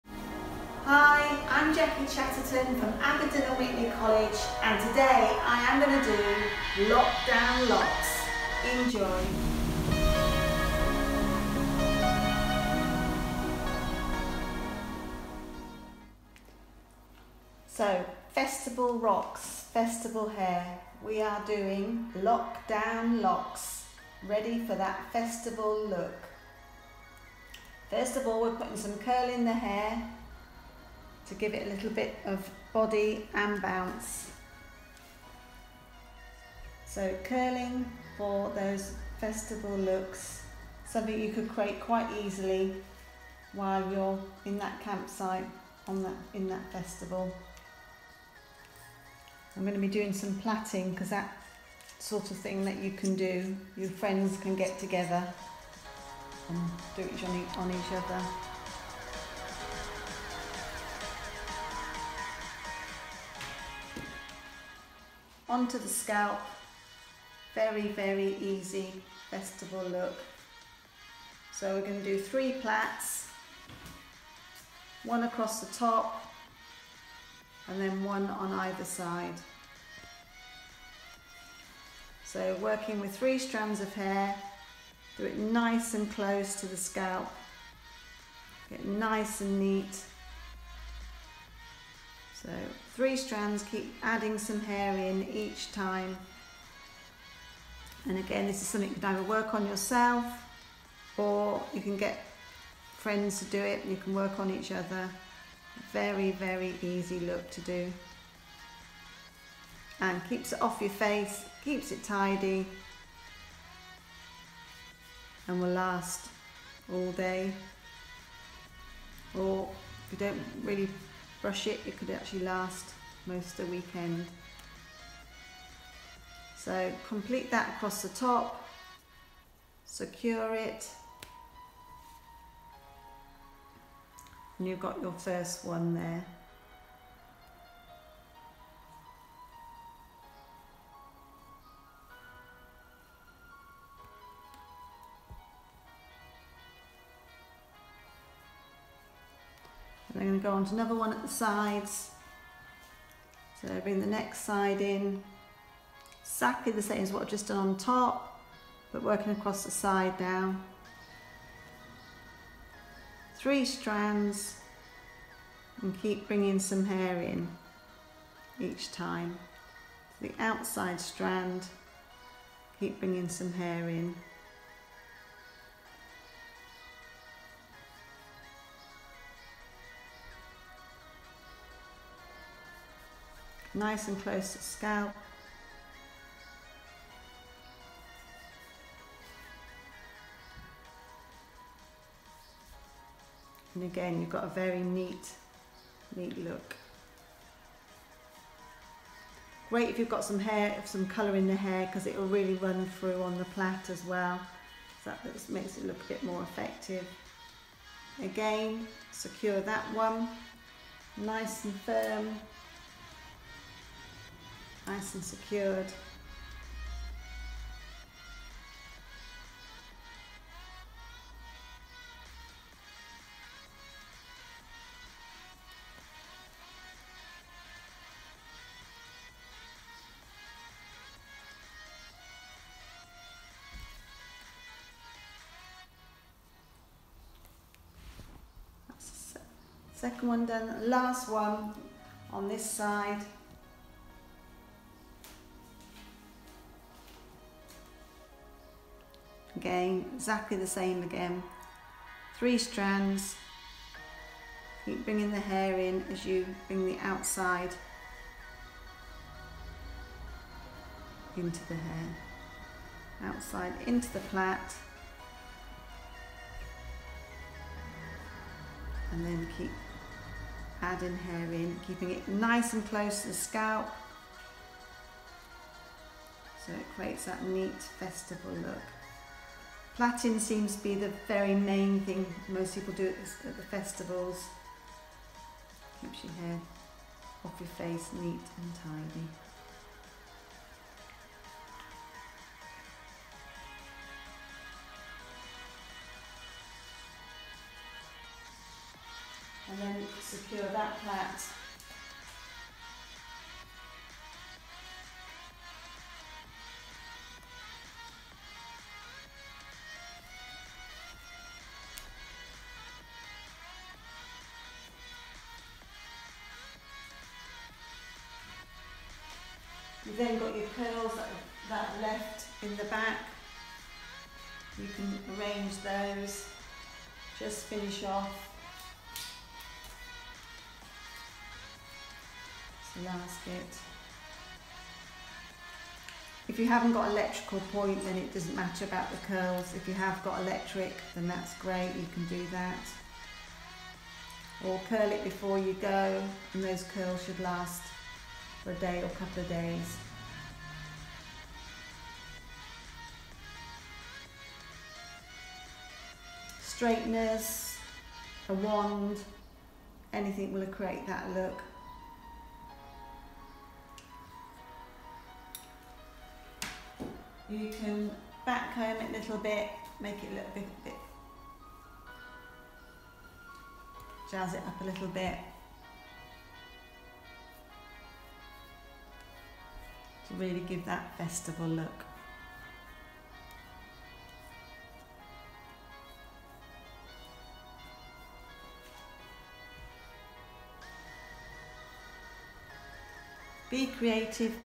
Lockdown Locks at JACKfest 2020
Abingdon & Witney College show us some lockdown locks tips for JACKfm's virtual festival. Shame it's of no use to Trev!

Relive all 8 hours of the full Facebook Live JACKfest